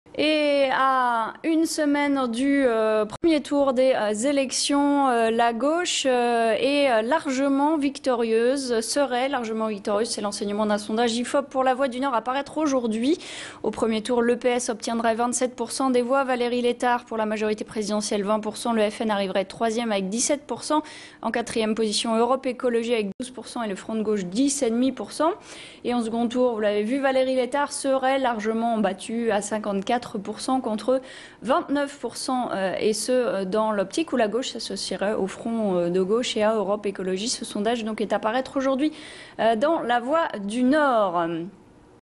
Régionales 2010 : Sondage Nord Pas-de-Calais
Sondage IFOP - La Voix du Nord à deux semaine des élections régionales dans le Nord Pas-de-Calais. (DROITS RESERVES . Pour toute exploitation sur TV, web, mobile, aller